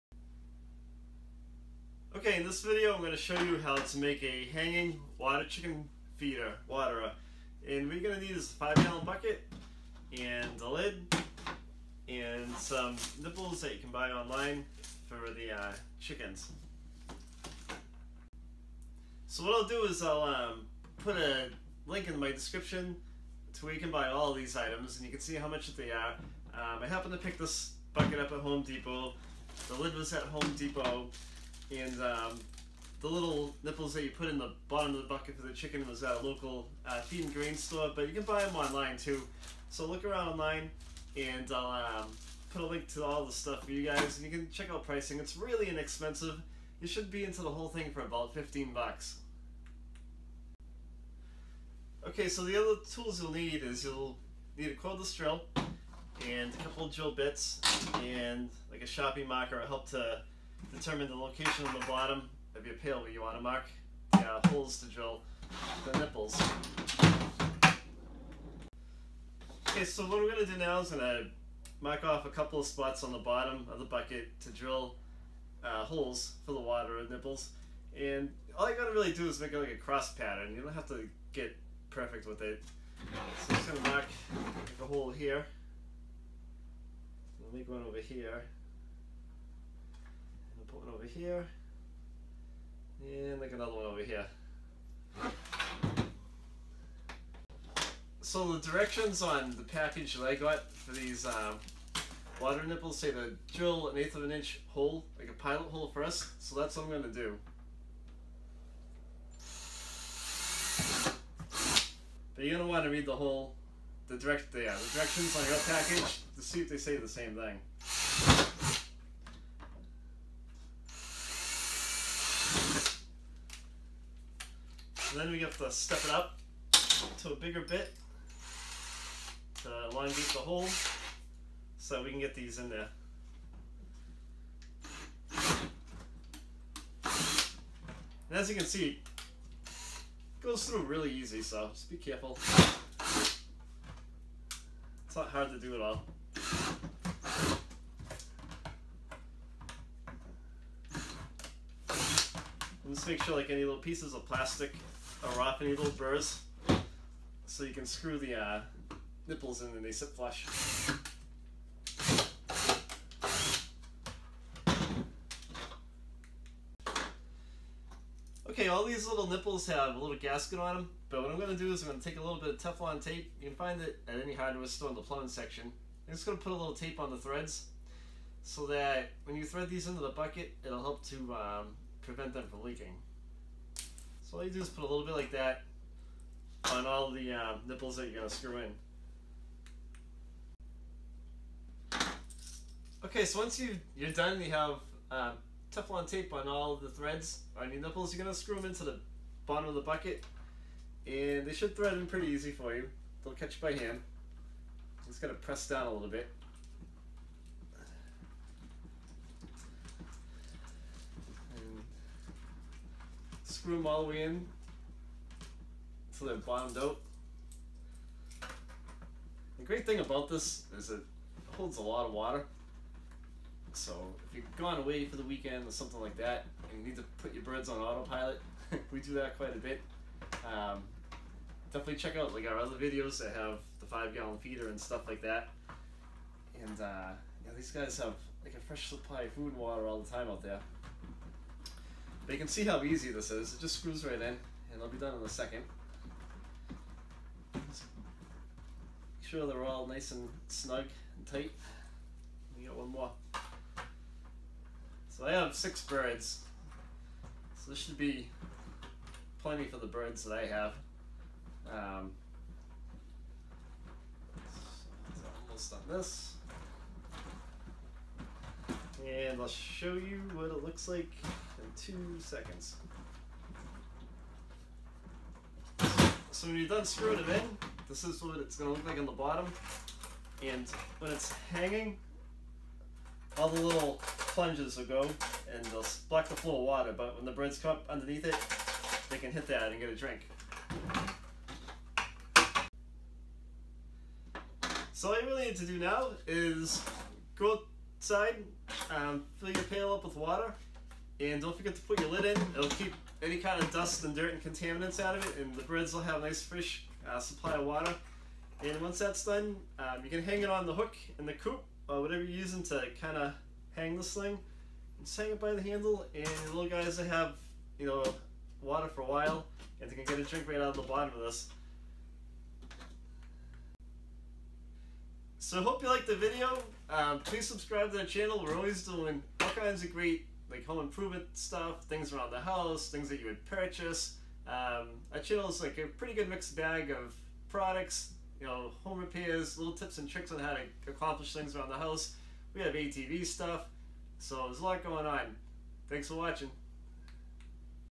How To Make a Hanging Chicken Waterer Using a 5 Gallon Bucket - Easy DIY
This video will show you how to make a hanging chicken waterer using a 5 gallon bucket. This is an easy DIY project. Total build time is about 10 minutes.

Parts List:

Home Depot 5 Gallon Bucket

Price $2.97

Home Depot Bucket Lid

Price $1.68

Chicken Waterer Nipples

Price $4.98